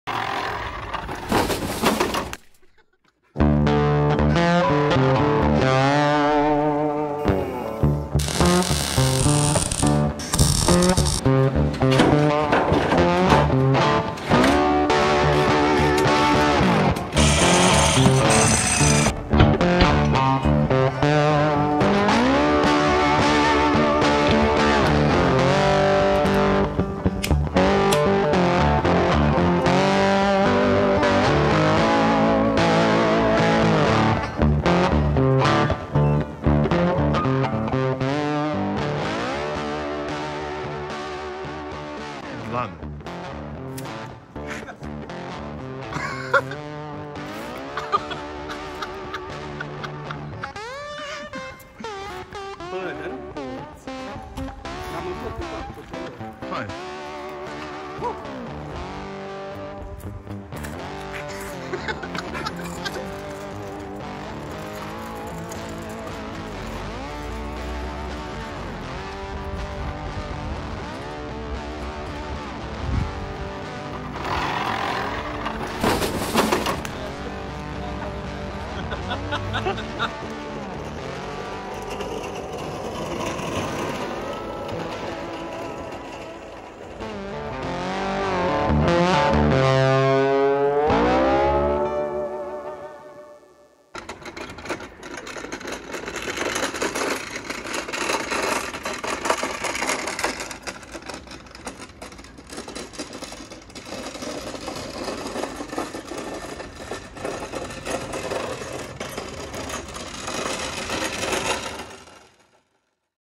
Electric rideable Beer Crate
We built this crazy electrified beer crate using a hoverboard found on the scrap heap!